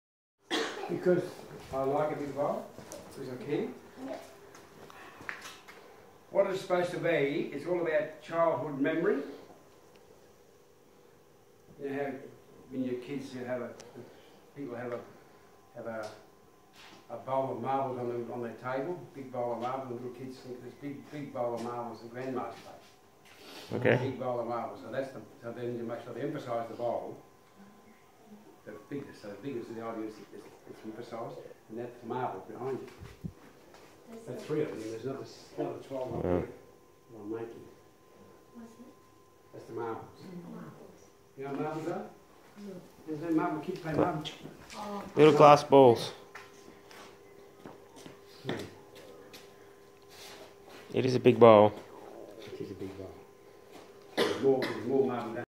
Australian Artist and sculptor John Abery showing wood sculpture - giant bowl of marbles part 1
Australian Artist and Sculpture John Abery showing his giant wooden bowl of marbles sculpture in the making.